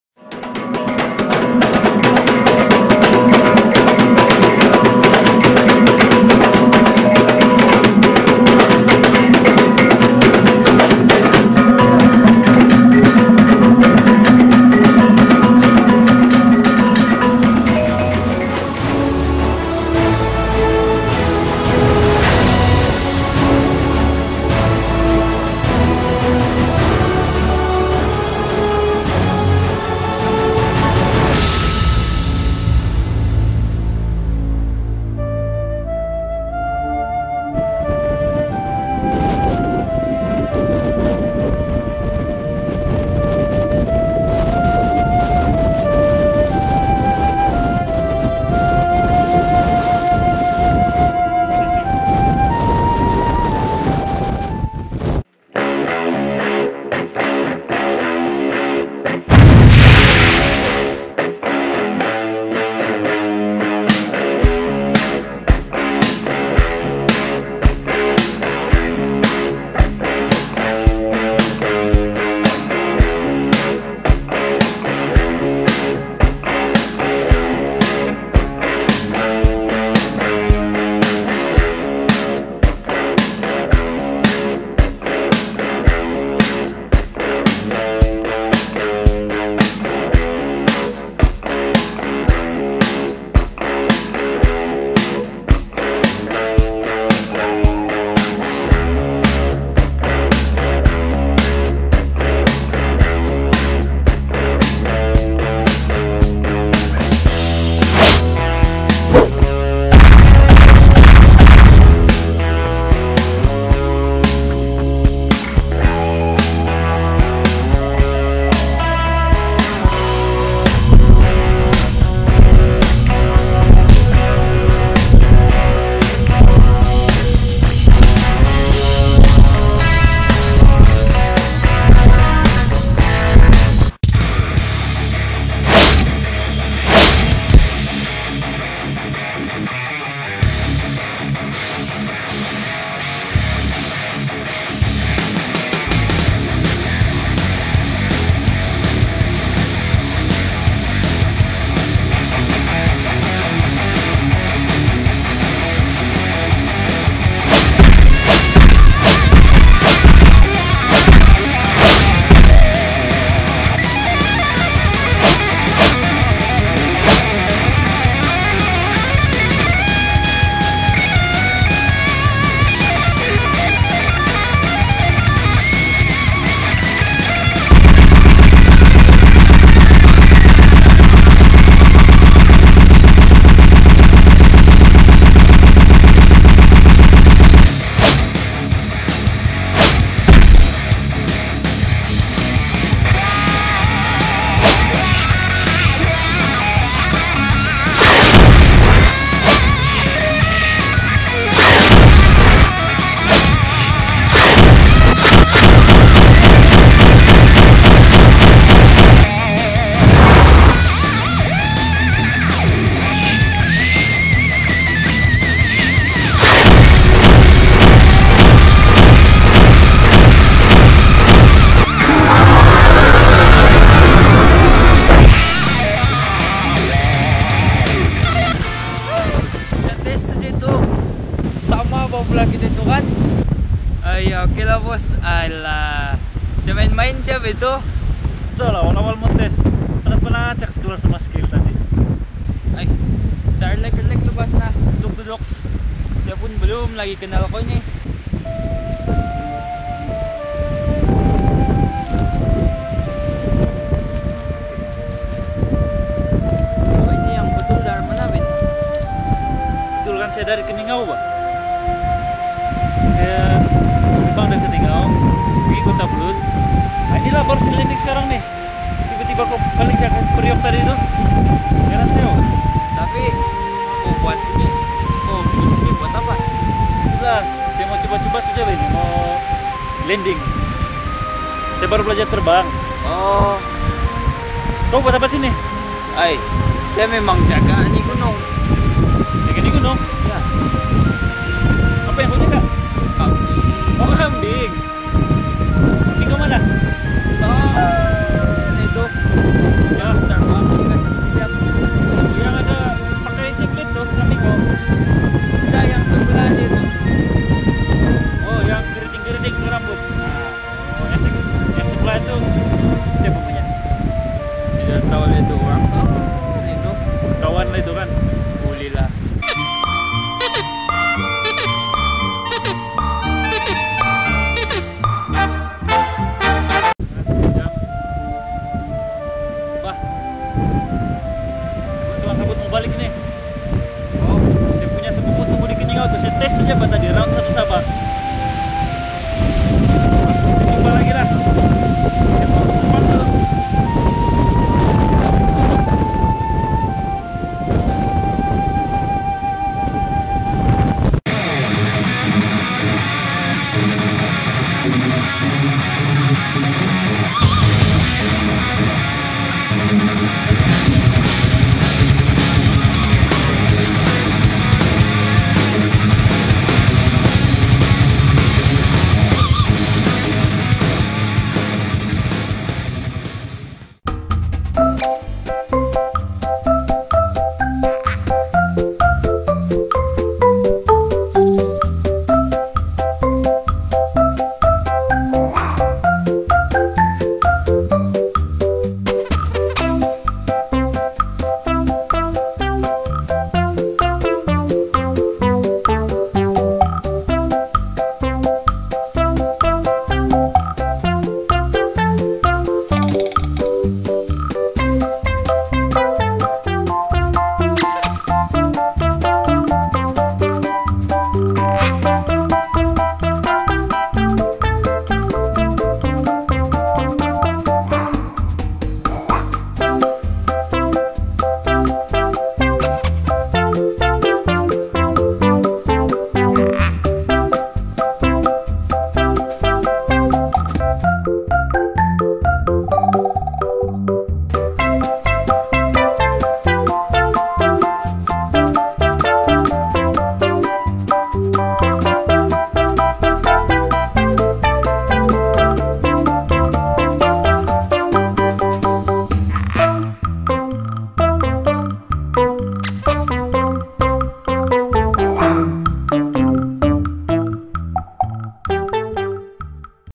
sony vegas effect test fighting effect dragonball
Pendekar Seluar Sendat vs Pendekar Lutut Berdarah.
Video singkat sesingkat duit gaji dalam dompet mengisahkan
"Pendekar Lutut Berdarah" baru belajar terbang lesen L mendarat di sebuah bukit yg dijaga oleh "Pendekar Seluar Sendat". berlakulah adegan test mengetest power Goku dragonball punya jiran. apa yang berlaku seterusnya, jum layan.